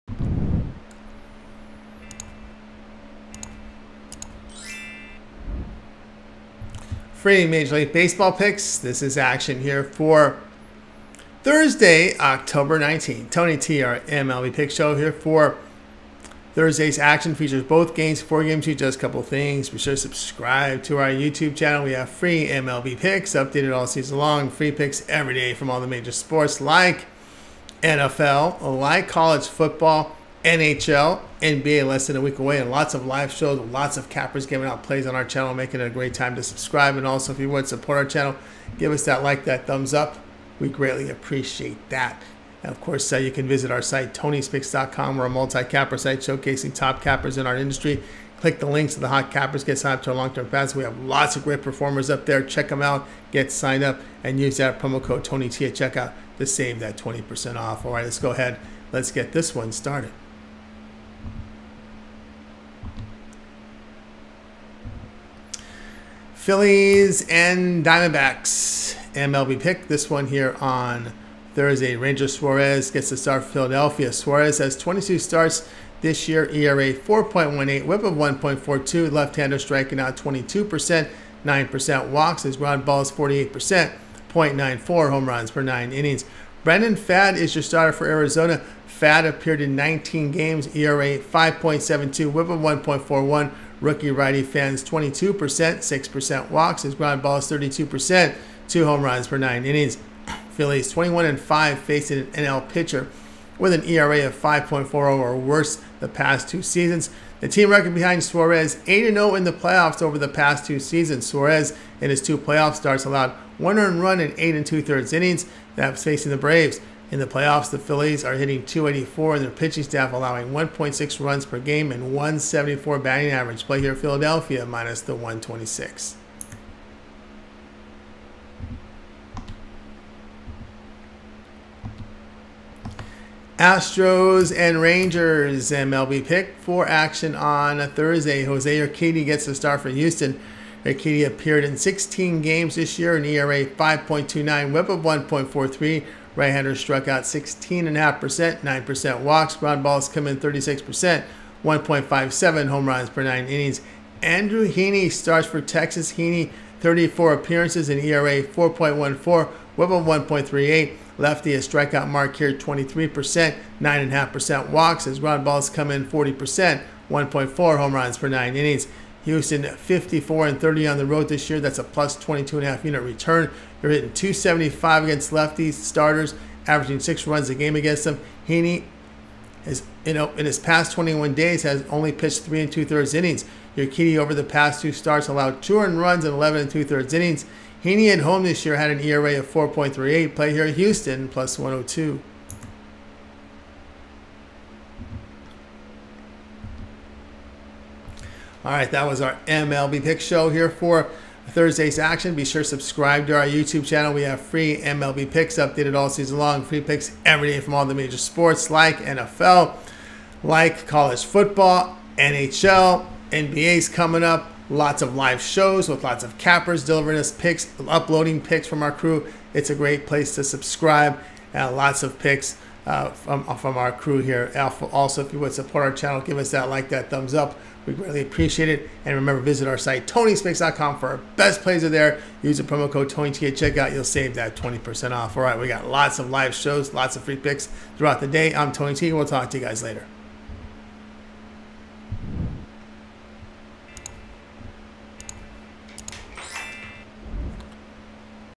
2 FREE MLB Picks and Predictions on MLB Betting Tips for Today, Thursday 10/19/2023
10/19/2023 FREE MLB Picks and Predictions on MLB Betting Tips for Today, Thursday

MLB GAMES FOR TODAY:
0:00 Intro
1:05 Philadelphia Phillies vs. Arizona Diamondbacks
2:13 Houston Astros vs. Texas Rangers